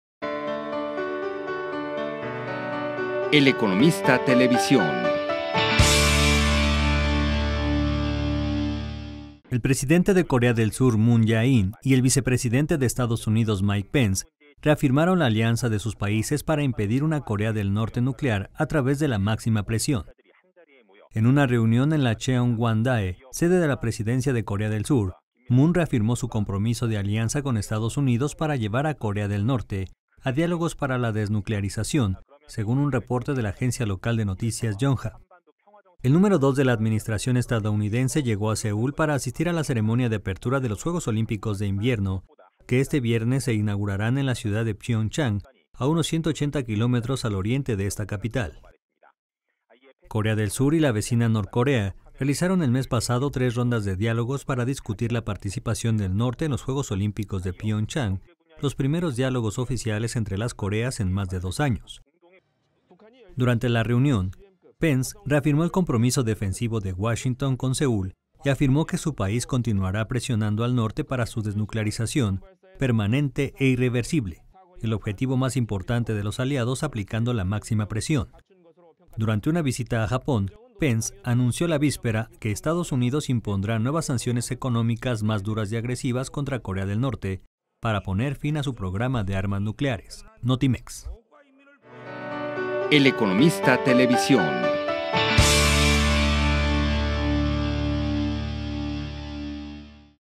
Corea del Sur y EUA aplicarán "máxima presión" a Corea del Norte
El presidente de Corea del Sur, Moon Jae-in, y el vicepresidente de Estados Unidos, Mike Pence, reafirmaron hoy la alianza de sus países para impedir una Corea del Norte nuclear a través de la "máxima presión".